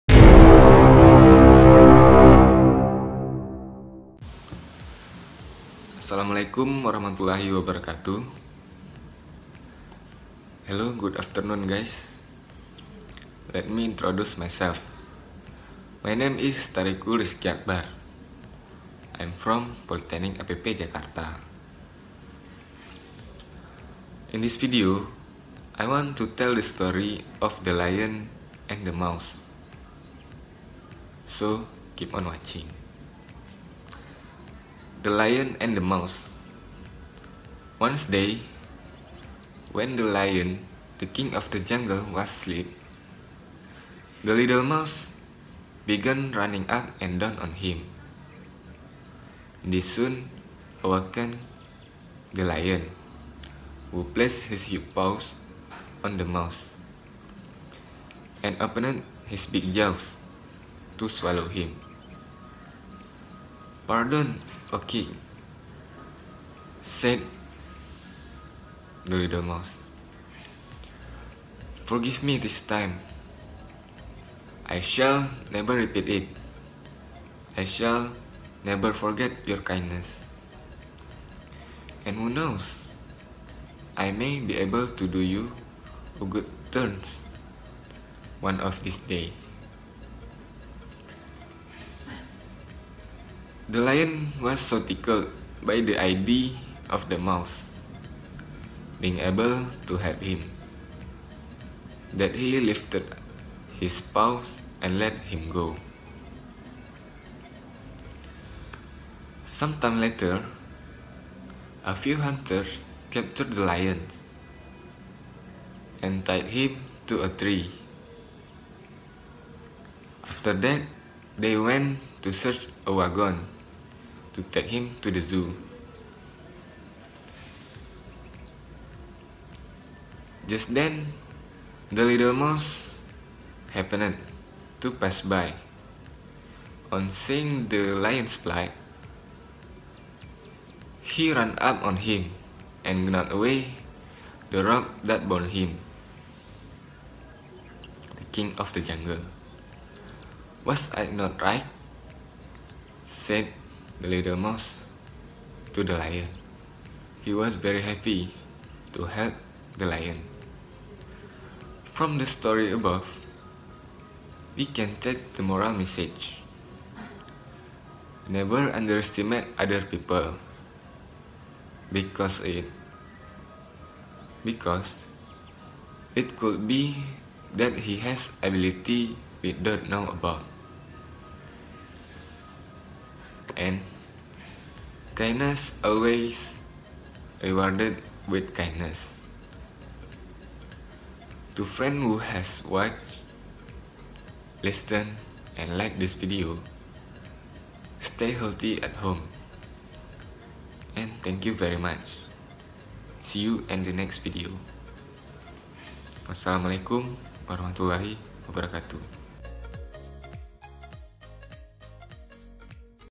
The Lion and The Mouse - Tariqul Rizky Akbar - MPIE2C1 - Public Speaking - PoltekAPP #2020
Tariqul Rizky Akbar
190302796
MPIE2C1
video ini dibuat untuk memenuhi Tugas UAS Public Speaking Prak